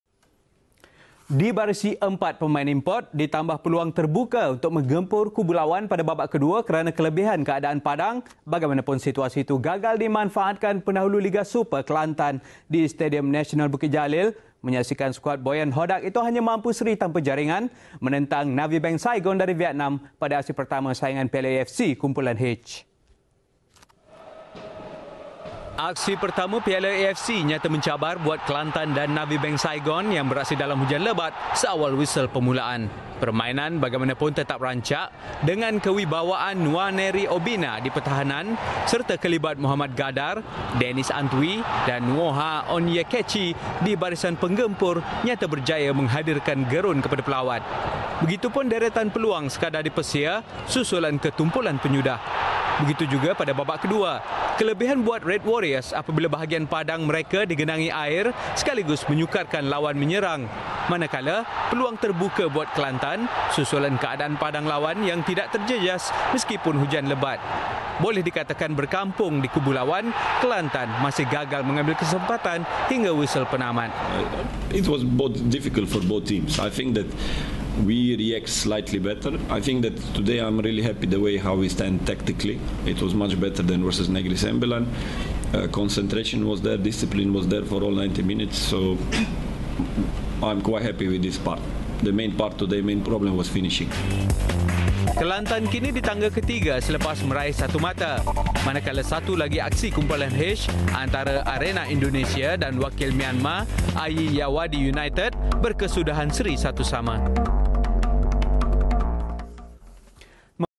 (HIGHLIGHT AFC CUP 2012) KELANTAN FA [0] VS [0] NAVIBANK SAIGON FC
FULL TIME : AFC CUP 2012 GROUP-H STAGE

KELANTAN FA [0] vs [0] NAVIBANK FC